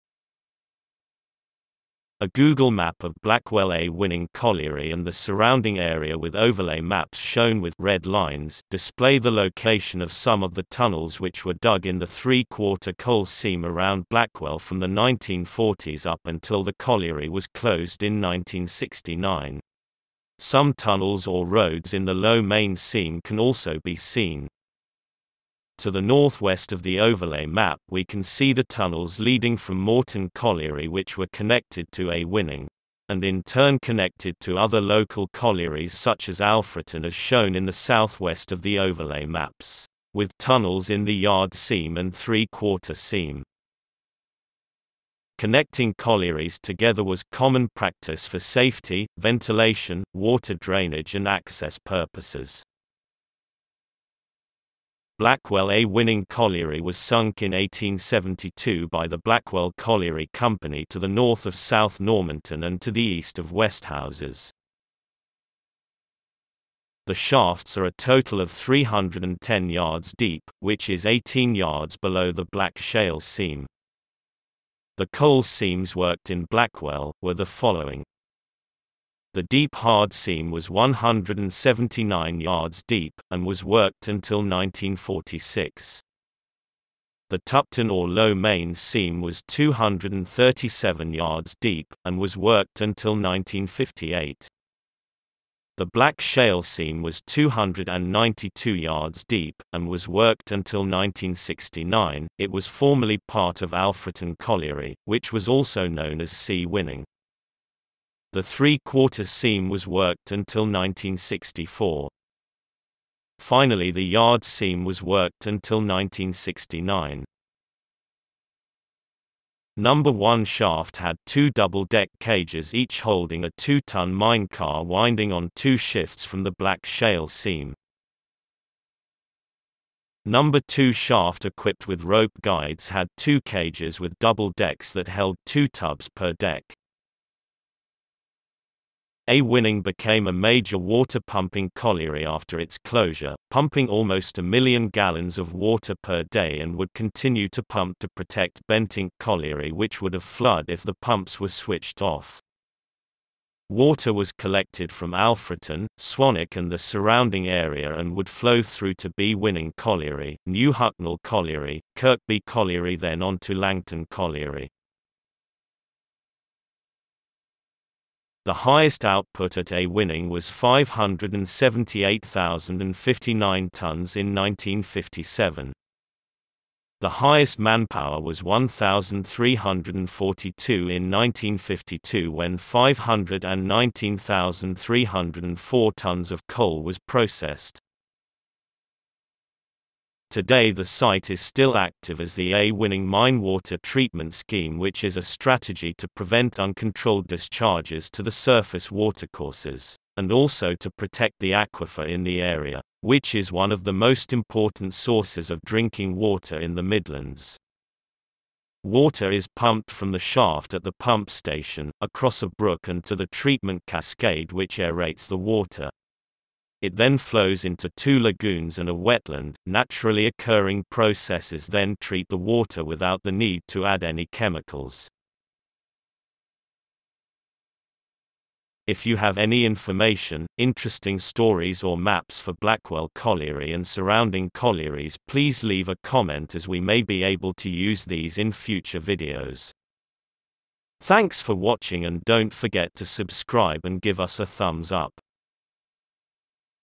Blackwell A Winning colliery overlay of coal mine tunnels in the Threequarter seam
A Google map of Blackwell AWinning colliery and the surrounding area with overlay maps shown with “red lines” display the location of some of the tunnels which were dug in the Three quarter coal seam around Blackwell from the 1940s up until the colliery was closed in 1969

some tunnels or roads in the low main seam can also be seen

To the north west of the overlay map we can see the tunnels leading from Morton colliery which were connected to AWinning

and in turn connected to other local collieries such as Alfreton as shown in the south west of the overlay maps

with tunnels in the Yard seam and Three quarter seam

Connecting collieries together was common practice for safety, ventilation, water drainage and access purposes

Blackwell AWinning colliery was sunk in 1872 by the Blackwell Colliery Company to the north of South Normanton and to the east of Westhouses

The shafts are a total of 310 yards deep, which is 18 Yards below the Black shale seam

The Coal Seams worked in Blackwell, were the following

The Deep Hard seam was 179 yards deep, and was worked until 1946

The Tupton or Low Main seam was 237 yards deep, and was worked until 1958

The Black shale seam was 292 yards deep, and was worked until 1969, it was formerly part of Alfreton colliery , which was also known as CWinning

The Three quarter seam was worked until 1964

Finally the Yard seam was worked until 1969

No 1 shaft had 2 double deck cages each holding a 2 ton mine car winding on 2 shifts from the Black shale seam

No 2 shaft equipped with rope guides had two cages with double decks that held 2 tubs per deck

AWinning became a major water pumping colliery after its closure, pumping almost a million gallons of water per day and would continue to pump to protect Bentinck colliery which would of flood if the pumps were switched off

Water was collected from Alfreton, Swanwick and the surrounding area and would flow through to BWinning colliery, New Hucknall colliery, Kirkbee colliery then onto Langton colliery

The Highest output at AWinning was 578,059 tons in 1957

The Highest manpower was 1,342 in 1952 when 519,304 tons of coal was processed

Today the site is still active as the AWinning mine water treatment scheme which is a strategy to prevent uncontrolled discharges to the surface watercourses
and also to protect the aquifer in the area,
which is one of the most important sources of drinking water in the Midlands

Water is pumped from the shaft at the pump station, across a brook and to the treatment cascade which aerates the water. It then flows into two lagoons and a wetland, naturally occurring pro-cesses then treat the water without the need to add any chemicals

If you have any information, interesting stories or maps for Blackwell colliery and surrounding collieries please leave a comment as we may be able to use these in future videos